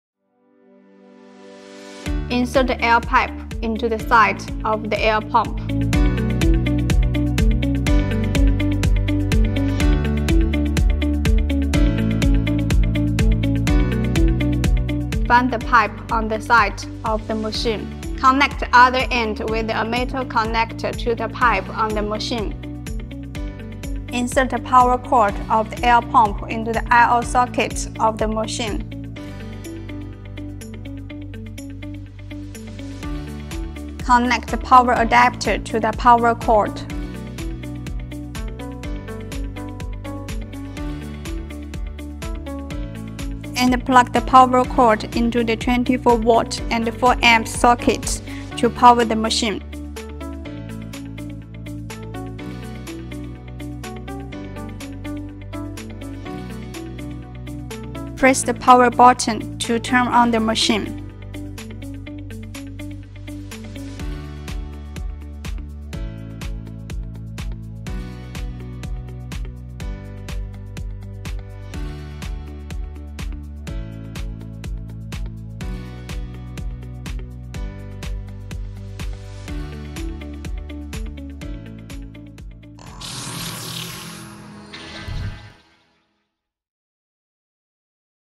How to connect the air pump | Delta Tutorial
In this video, we will guide you on how to connect the air pump for Delta.
1.Insert the air pipe into the side of the air pump
2.Find the pipe on the side of the machine, connect the other end with a metal connector to the pipe on the machine
3.Insert the power cord of the air pump into the I/O socket of the machine
4.Connect the power adapter to the power cord